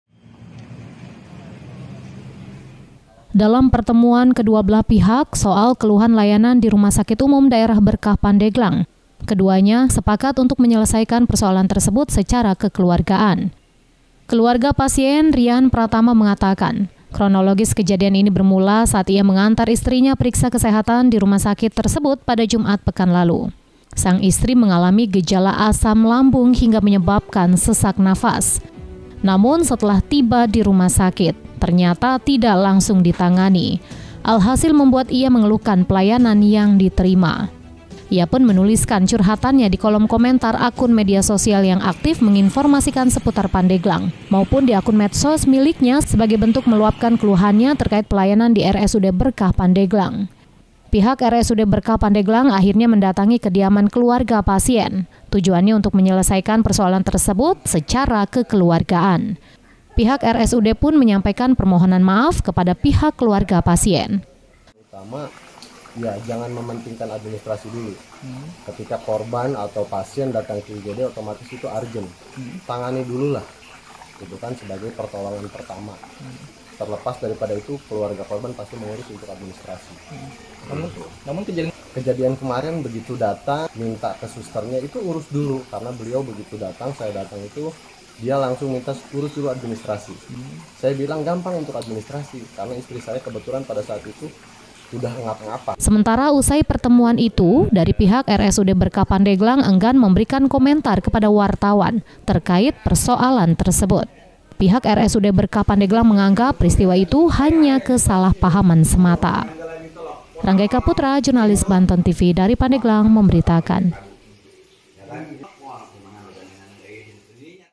KELUHAN KELUARGA PASIEN RSUD BERKAH DISELESAIKAN SECARA KEKELUARGAAN
Pasca viral curhatan warga Pandeglang di media sosial soal keluhan layanan di Rumah Sakit Umum Daerah Berkah Pandeglang, pihak keluarga pasien dan RSUD Berkah Pandeglang bertemu untuk menyelesaikan persoalan tersebut secara kekeluargaan.